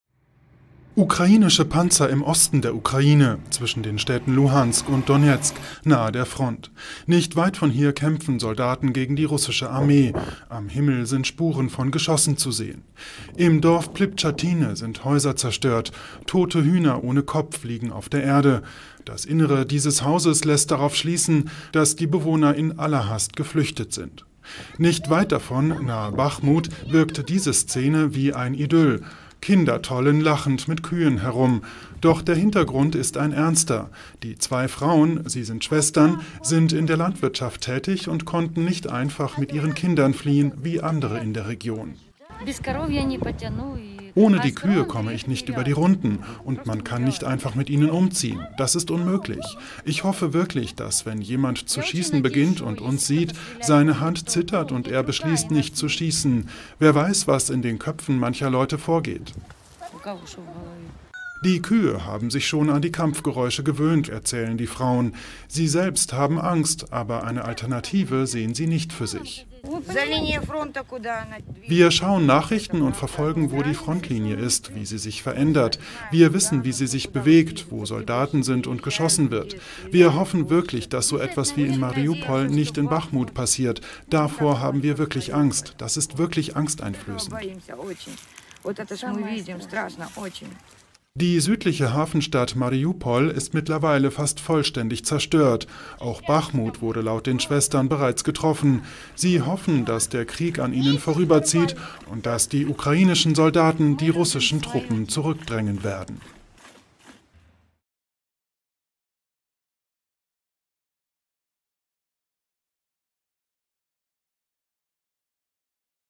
Mit Milchkühen nahe der Front: Familien harren in der Ost-Ukraine aus | AFP
Trotz Kämpfen in der Ostukraine flüchten einige Menschen nicht aus der Region - zum Beispiel, weil sie in der Landwirtschaft tätig sind und ihre Milchkühe nicht zurücklassen wollen, so wie die Melkerinnen Oksana But und Ljudmyla Dynalewskaja mit ihren Kindern.